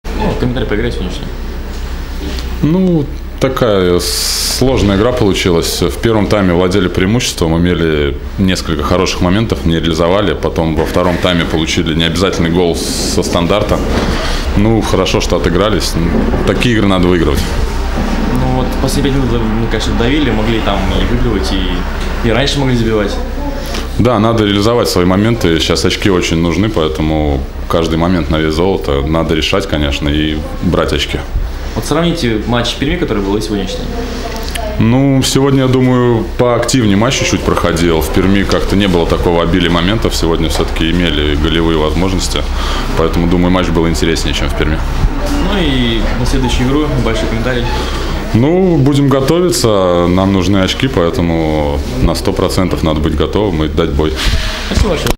Интервью Дмитрия Белорукова после матча «Уфа» — «Амкар» 1:1 [19 тур РФПЛ]
«Уфа» - «Амкар» (Пермь) - 1:1

Голы: Шевченко, 72 – Прудников, 85,
Судья: С. Карасев (Москва)
14 марта. Саранск. Стадион «Старт».